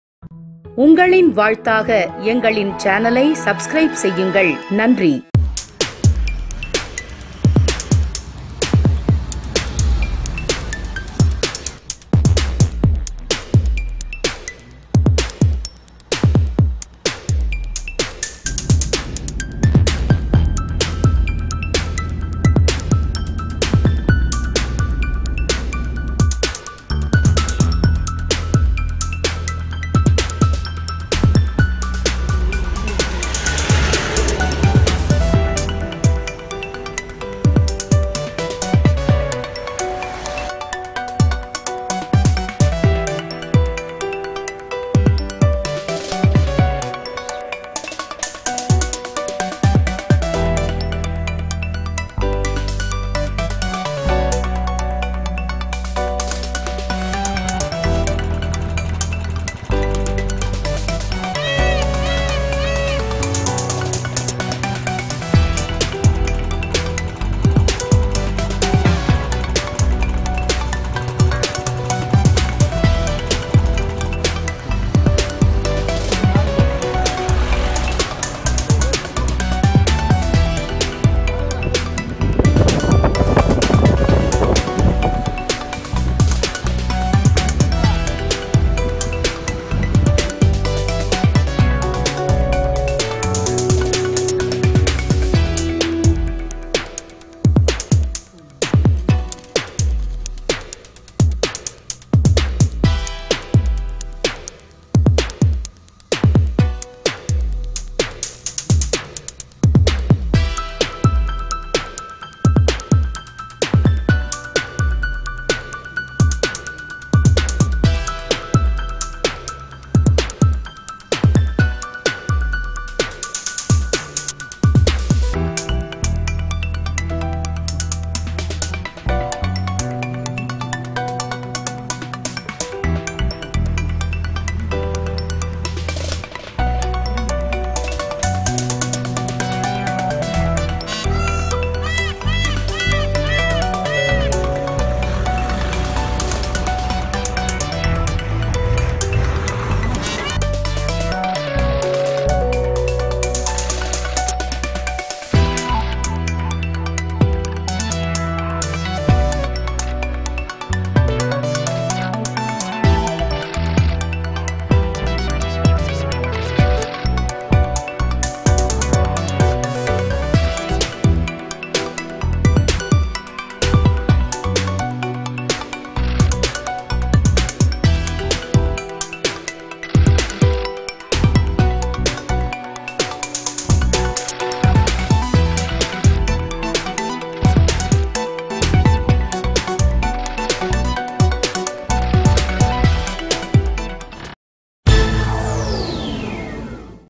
🌄முதுமலையின் அழகு காட்சிகள்
Mudumalai National Park is a declared tiger reserve that is located in the northeast of Nilgiri Mountain or the blue mountain. It is located in Nilgiri district and shares its borders with Kerala and Karnataka. The park is a home to numerous endangered species like Bengal tiger, long billed vulture, white-rumped vulture, Indian leopard, Indian elephant and others.